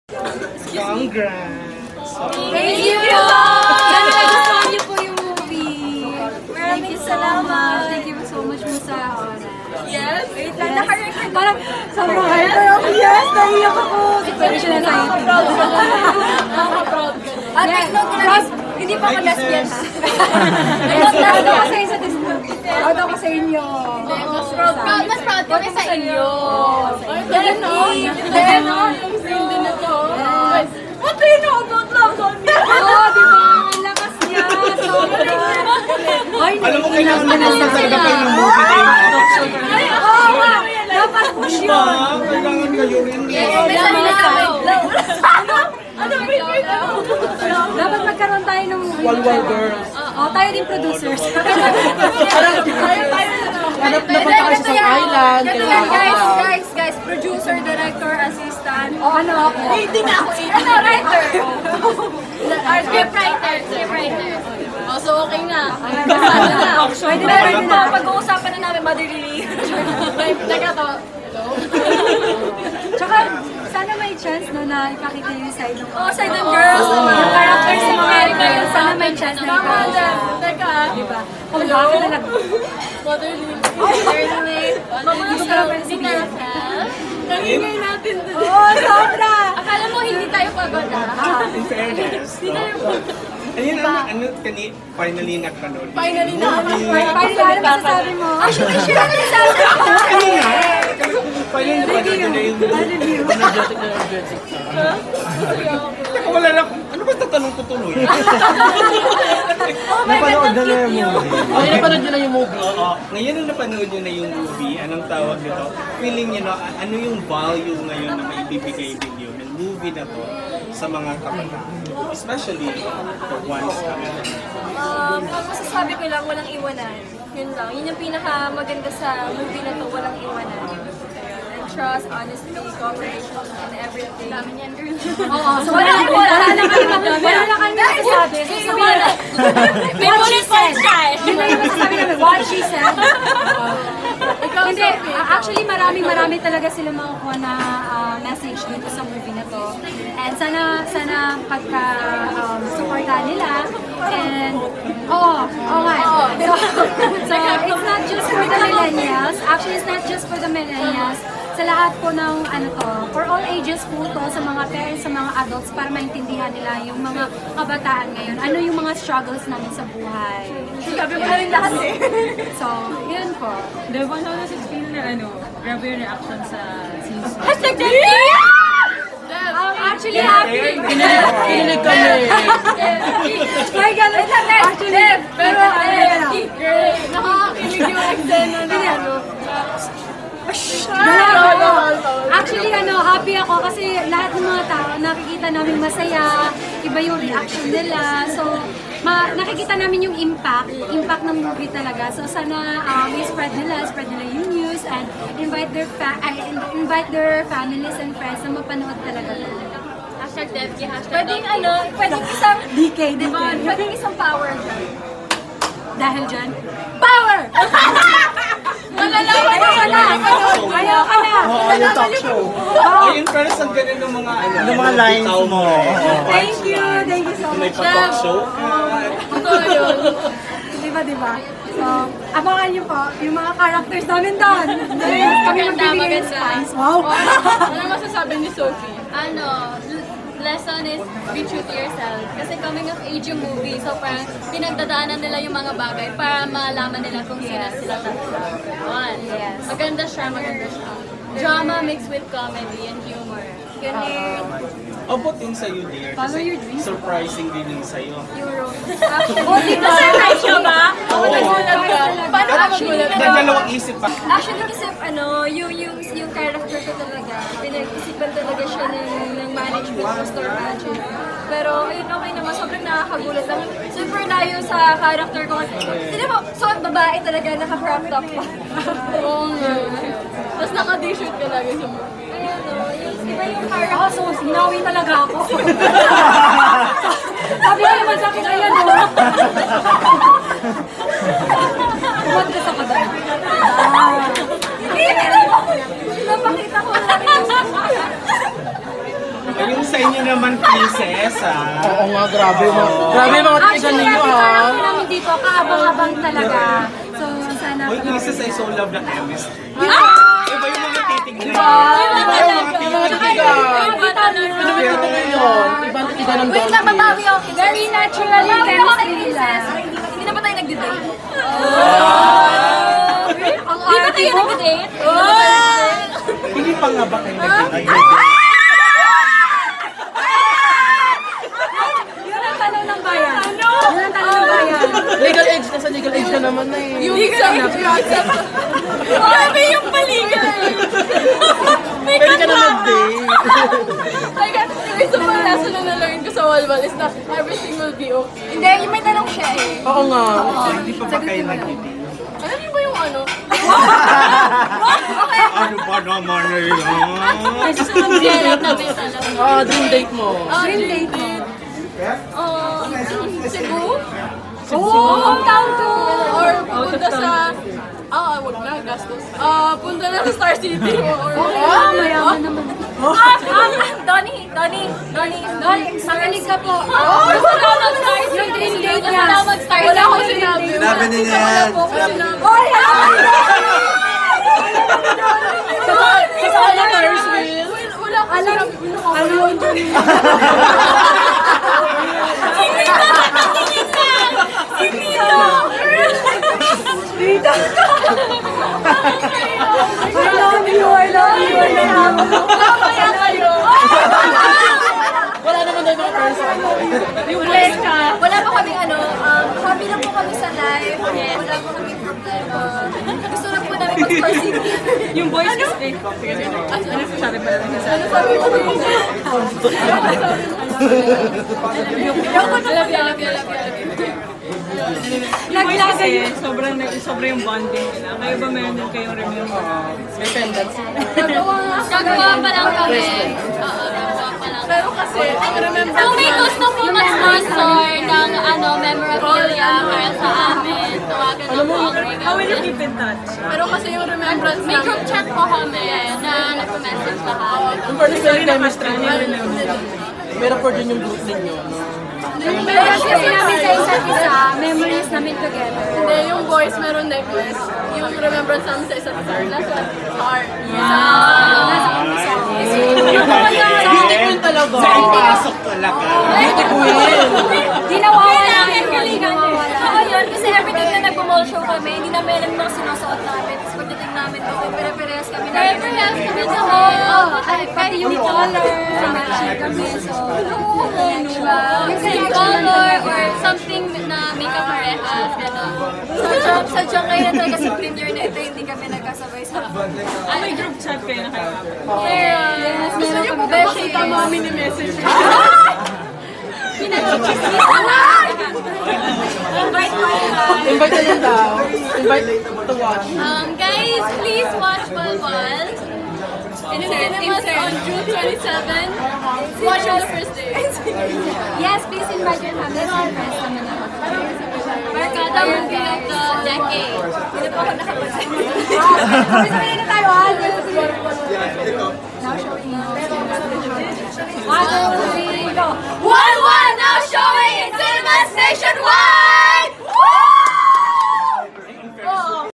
Kisses Delavin | Devon Seron | Jane De Leon | Reaction After Watching Their Film Walwal
Kulitan with Kisses Delavin, Devon Seron, Jane De Leon, Sophia Senoron After The Red Carpet Premiere of their film Walwal

Walwal
Starring: Elmo Magalona, Kiko Estrada, Jerome Ponce, Donny Pangilinan, With Devon Seron, Kisses Delavin, Jane De Leon, Sophia Senoron
Directed by Jose Javier Reyes
June 27 in Cinemas Nationwide